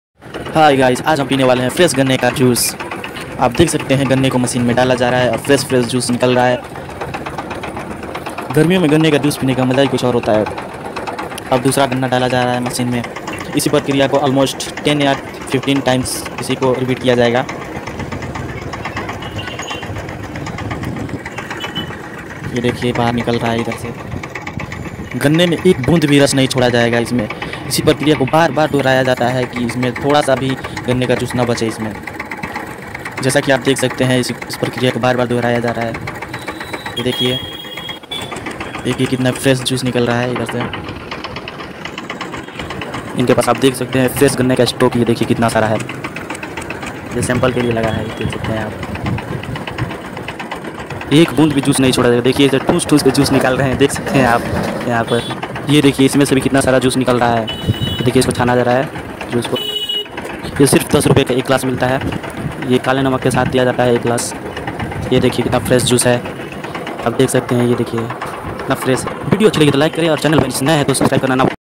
Fresh ganne ka juice | only 10 rupees | enjoy summer season with juice of Ganna
Mr Aaron vlog